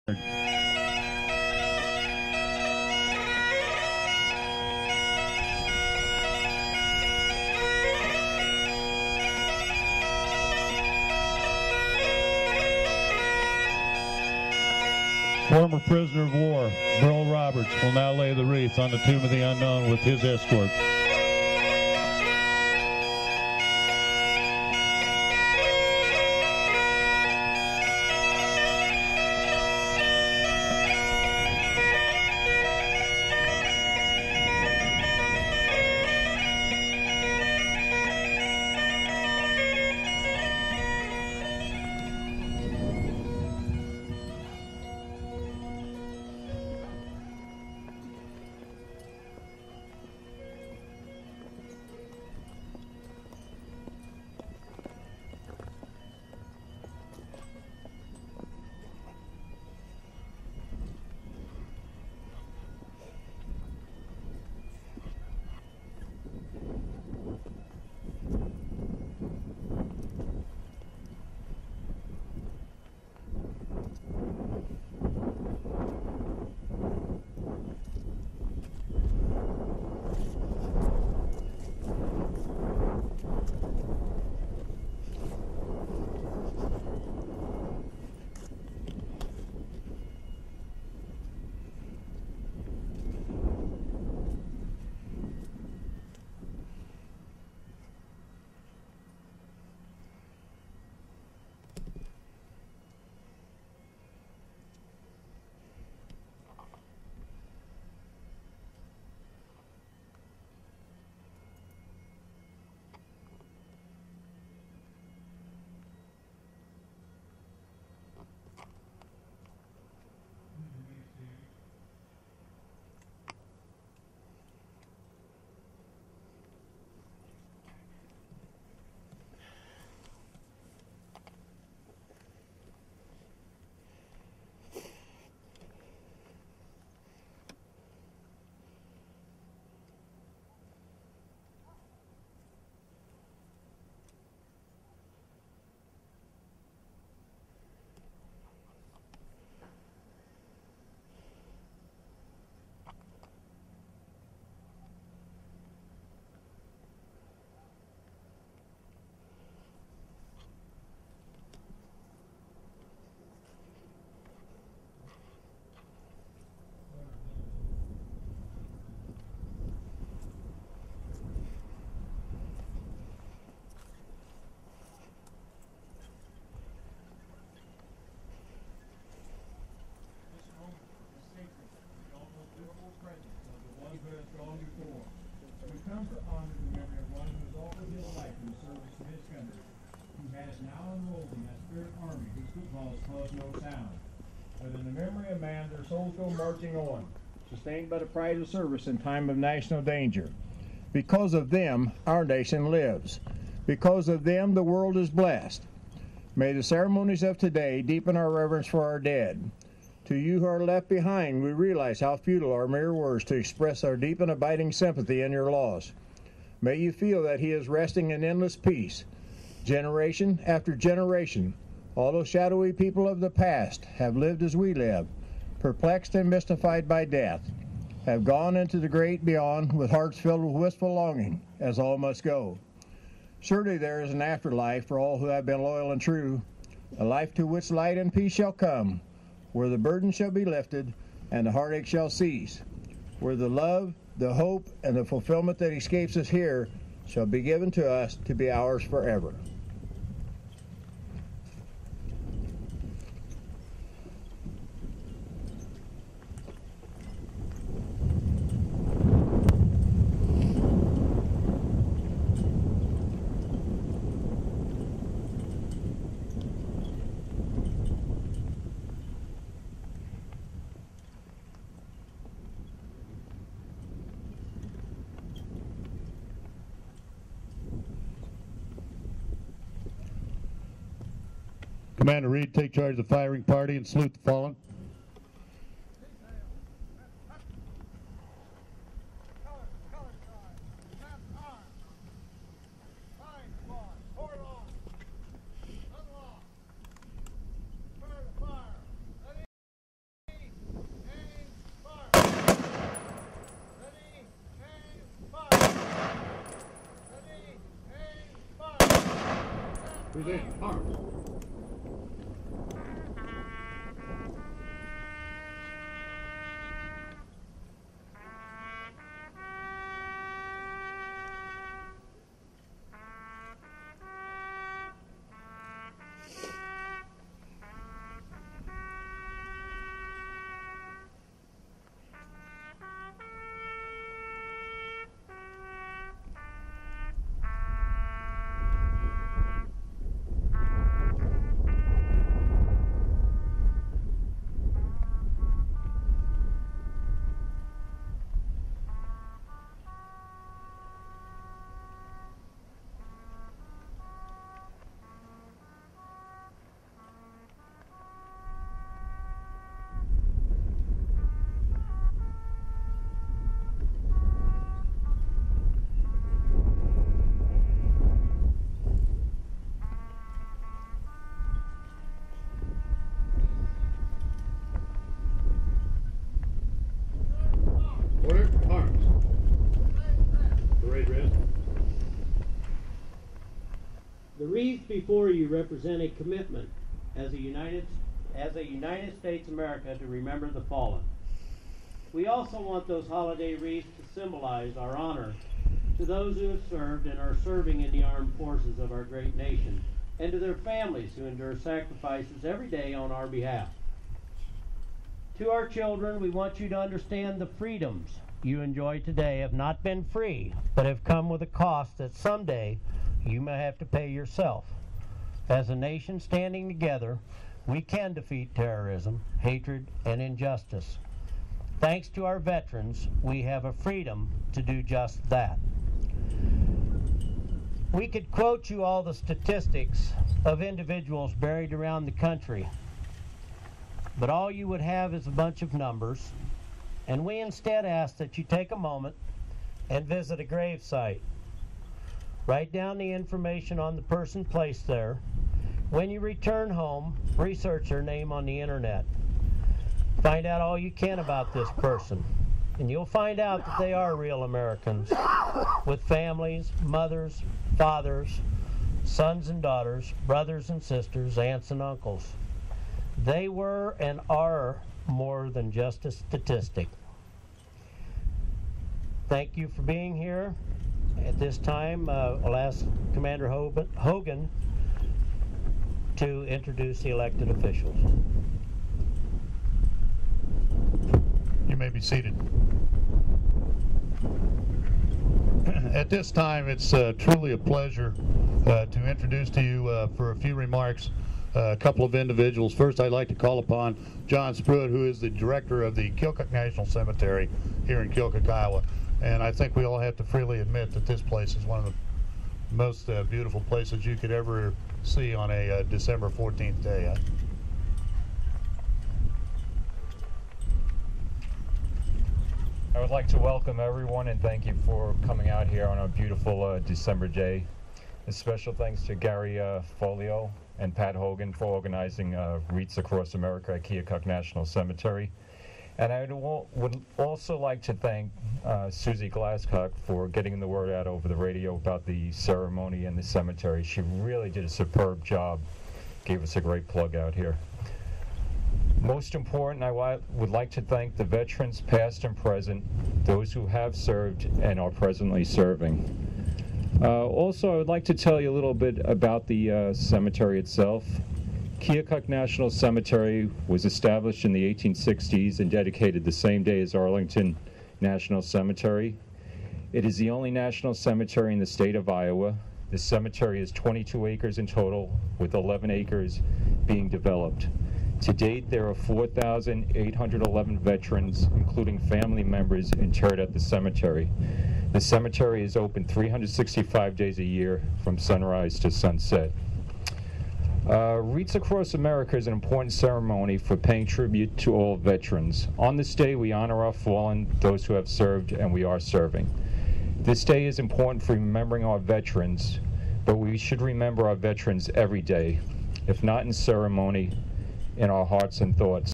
Wreaths Across America - Keokuk pt 2
The nationwide Wreaths Across America ceremony honoring our veterans, Dec. 14, 2006. This video is from Keokuk, IA, National Cemetery and courtesy of Golden Point Video.